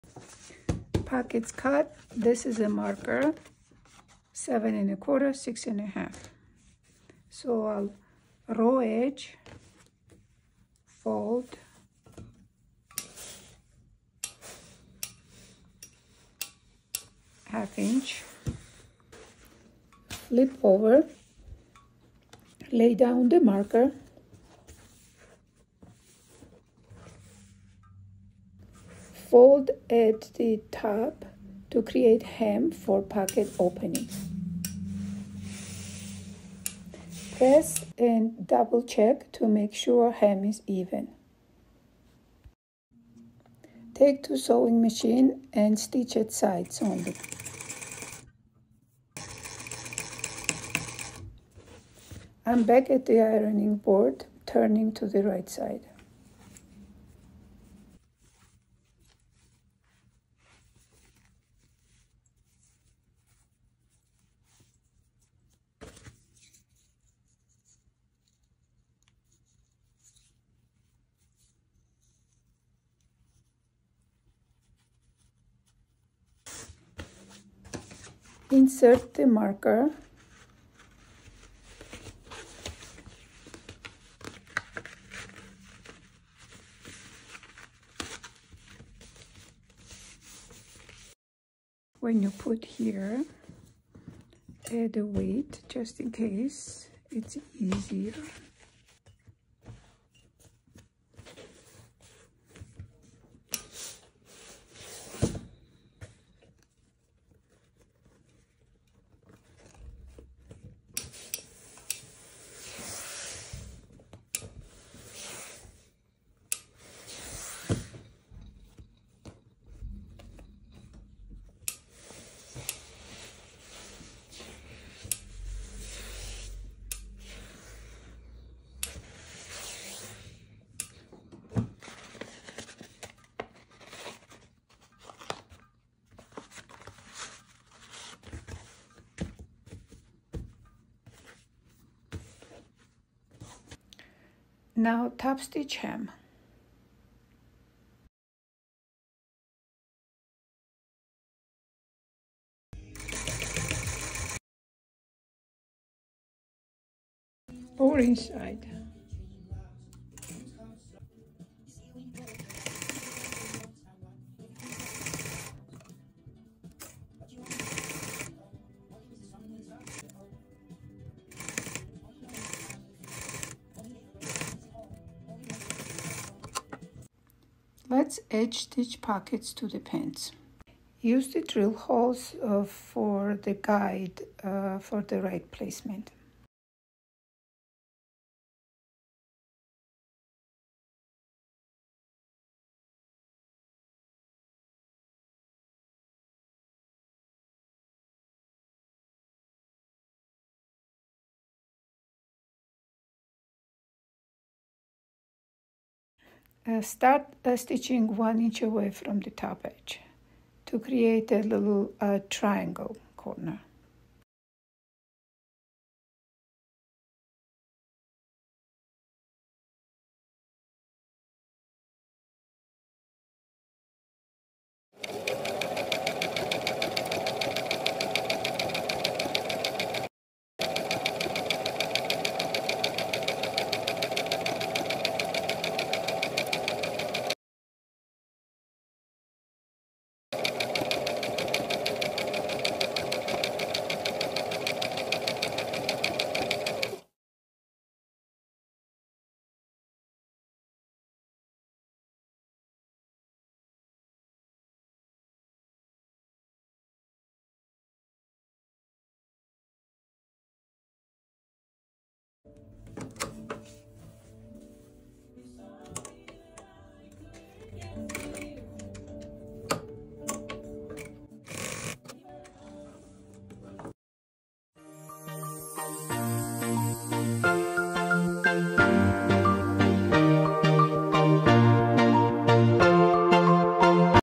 Patch Pocket Sewing
Even simple pack pocket needs certain technique, especially in fabrics that are soft, like this organic linen.